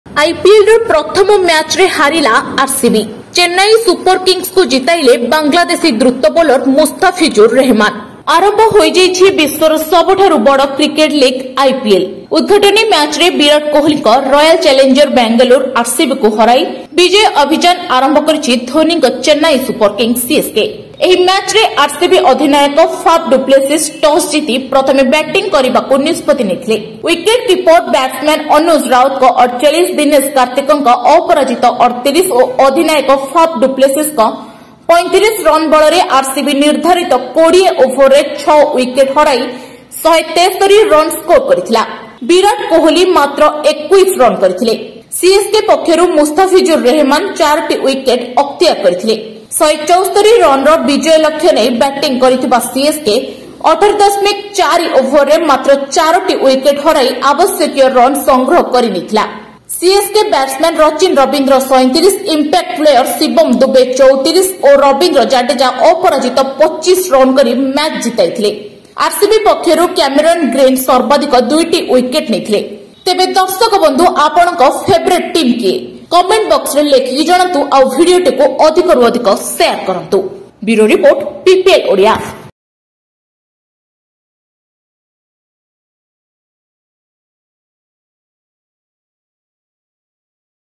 Bangladeshi fast bowler Mustafizur Rahman beat Chennai Super Kings | PPL Odia
ଆଇପିଏଲ୍ ର ପ୍ରଥମ ମ୍ୟାଚ୍ ରେ ହାରିଲା RCB  ଚେନ୍ନାଇ ସୁପର କିଙ୍ଗ୍ସ କୁ ଜିତାଇଲେ ବାଂଲାଦେଶୀ ଦ୍ରୁତ ବୋଲର୍ ମୁସ୍ତାଫିଜୁର ରେହେମାନ |